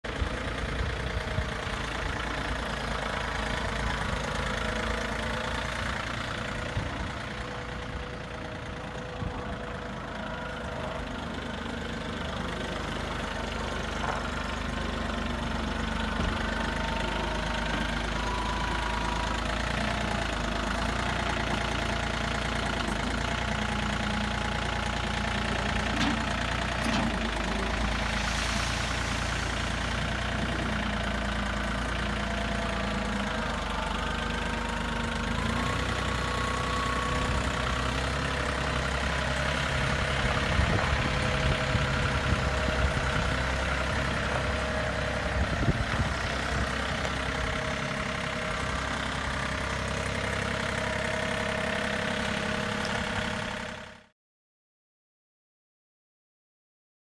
Köp Kompakttraktor John Deere 3520 med lastare och redskap på Klaravik
Kompakttraktor John Deere 3520 med lastare och redskap
Fabrikat: John Deere
Modell: 3520
Fordonsår: 2007
Chassienr / serienr: LV3520E370051
Mätarställning: 2055 h
Motoreffekt (kw/hk): 27 kW

Drivmedel: Diesel

Fordonsstatus: Itrafik

CE-märkt: Ja

Utrustad med:
- 4WD
- Lastare
- Frontlyft
- Fronthydraulik
- Ktaftuttag fram
- A/C

- Sopvals
- Sandspridare
- Skopa
- Pallgafflar
- Bakvikt
- A-rams fäste

Tjänstevikt (faktisk vikt): 1800 kg
Max lastvikt: 1380 kg
Totalvikt: 3180 kg